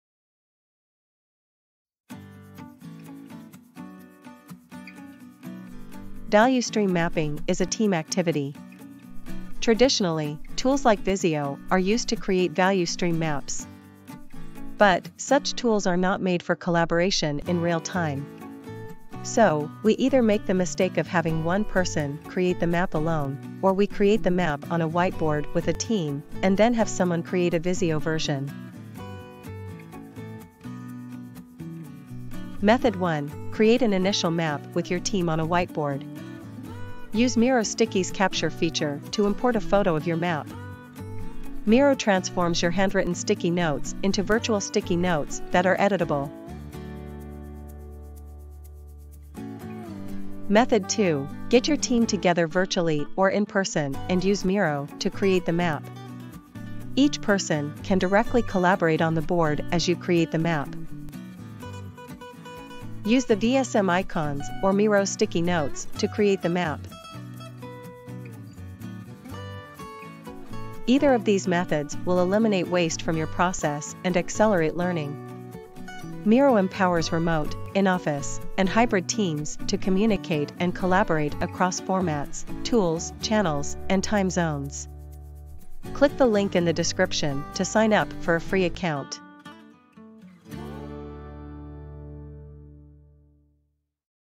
Value Stream Mapping Software: How to Use Miro for Value Stream Mapping Events
Value Stream Mapping is a team activity.  What usually happens is a Value Stream Map is created by a team on a whiteboard using stickie notes, and then someone manually recreates the map, adding wasteful steps.  If the team makes changes, they are made on the whiteboard and then someone goes back and updates the software tool.  Do you see a problem with this?  It is extremely wasteful.  Let’s talk about Miro, an online system that empowers remote, in-office, and hybrid teams to communicate and collaborate across formats, tools, channels, and time zones, without the constraints of physical location, meeting space, and whiteboards.   With Miro, there are two different ways you can facilitate and create Value Stream Maps without the wasteful step of recreating them.

Method 1: Create an initial map with your team on a whiteboard or wall using stickie notes.  Then, use Miro’s “Stickies Capture” feature to import a photo of your map.  Then, the magic happens.  Miro transforms your handwritten sticky notes into virtual sticky notes that are editable.  You can then use the Miro document going forward.  All of your team members can edit directly from Miro, and there is no need to update both copies.

Method 2: Get your team together virtually or in-person and use Miro to create the map.  Each person can directly collaborate on the board as you create the map.  There are at least two alternatives.  If your team is familiar with traditional VSM icons, you can use the Value Stream Mapping icons to create a map.  If your team prefers using sticky notes, you can use Miro’s sticky notes to create the map.  You can then do your brainstorming and even teaching of key concepts within Miro if you prefer.  The team can then create the Future State Map and Action plan using Miro’s templates or a template you create yourself.
Either of these methods will eliminate waste from your process and accelerate learning.

and sign up for a free account.